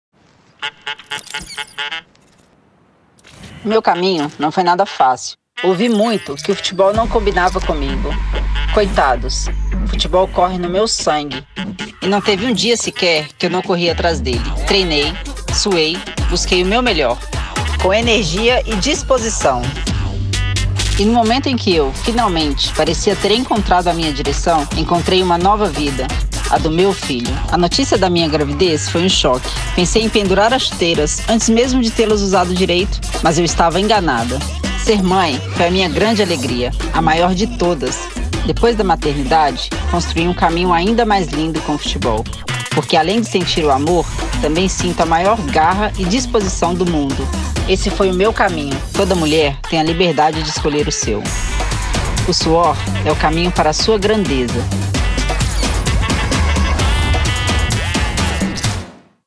Tamires | O suor é o caminho
A Tamires é craque quando o assunto é surpreender o destino e traçar o próprio caminho. Mesmo quando o destino a surpreende primeiro.

Solta o dedo no play e vem descobrir com a gente essa história de amor pelo esporte e pela família. 🧡⚡